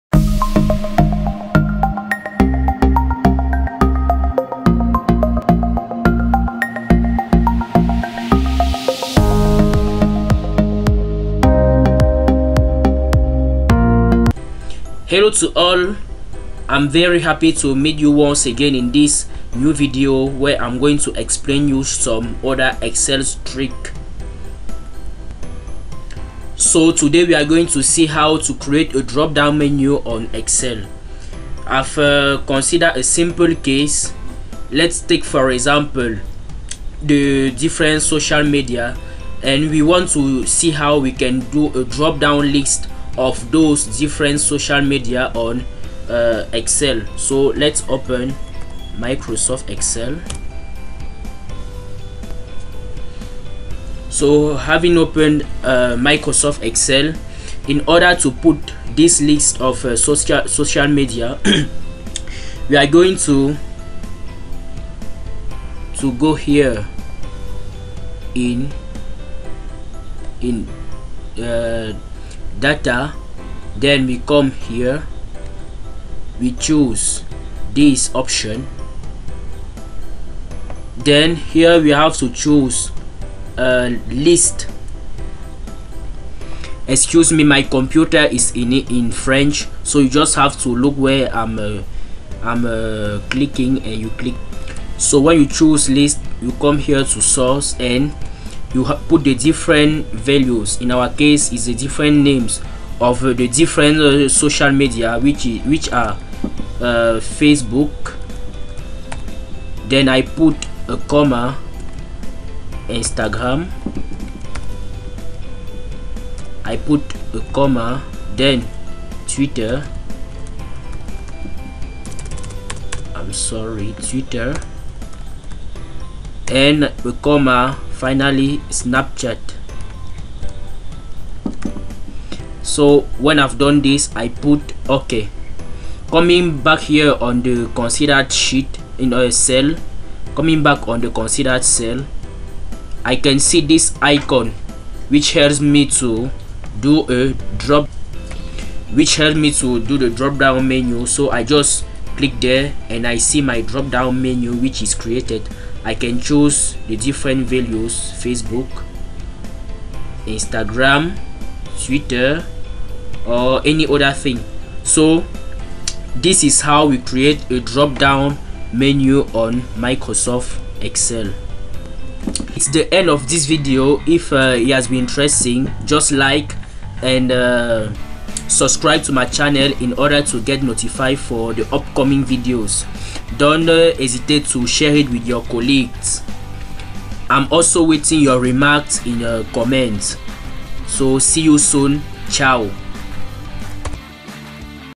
How to Create a Drop down menu in Microsoft Excel
This video explains how we can create a drop down menu on Ms Excel.